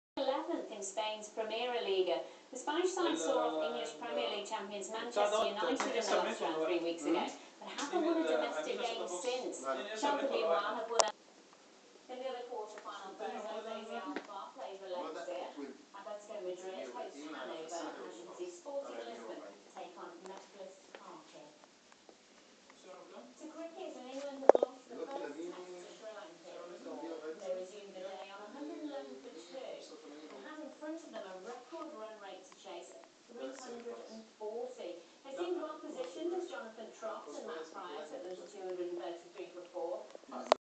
Customized Mirror TV system by iNTEGRA
Mirror TV project done for a 5 Star Hotel in Dubai. Project consisted of designing, producing and installing units for every suite. The system consists of a 42inch Hotel Interactive TV with a FeONIC drive that converts the Mirror into a speaker.